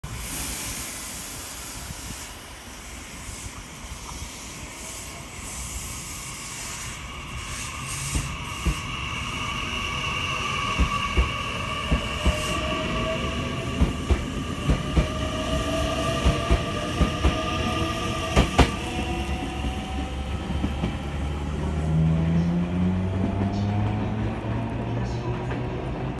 12/1 祝！デビュー【名鉄9500系】SiC VVVF！9501F 急行吉良吉田行 豊明発車
豊明21:43発 急行吉良吉田行 土休日2142レ
9501F
佐屋20:41発 普通吉良吉田行 須ヶ口から準急 名古屋から急行 吉良吉田22:24着